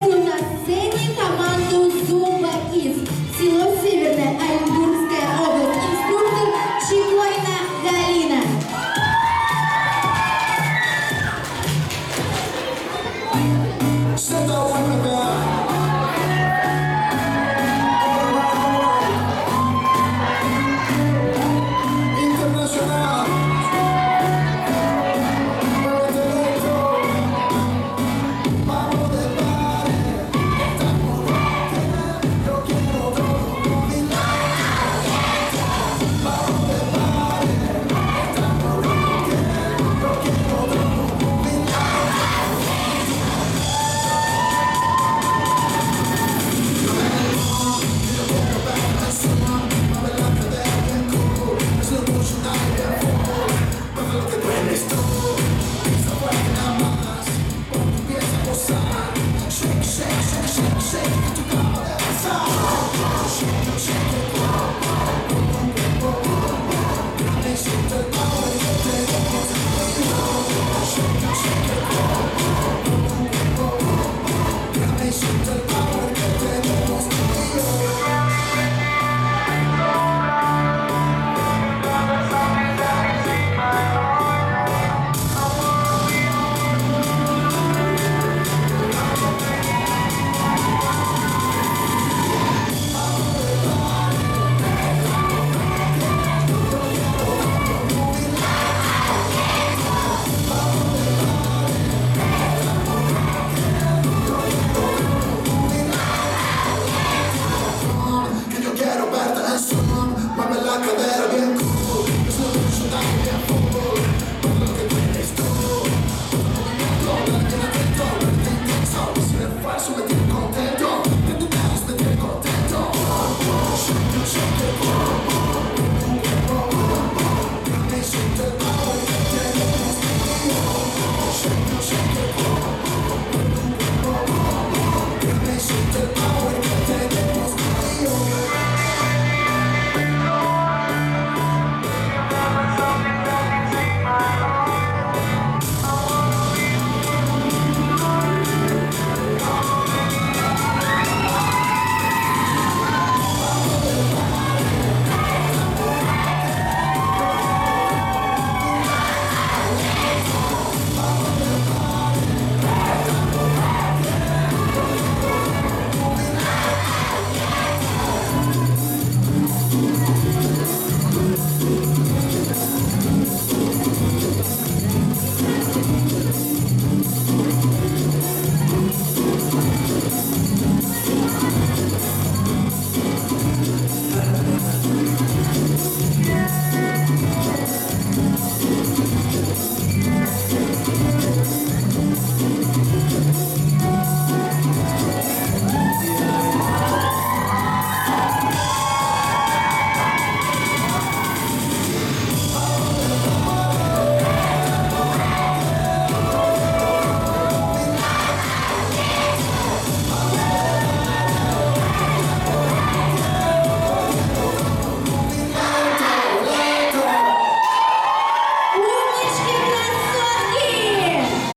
Zumba kids Северное Чекорина Галина
МБУК ЦКД "Восход"
🏆13 апреля состоялся
I Межрегиональный танцевальный фитнес фестиваль "Я-танцую"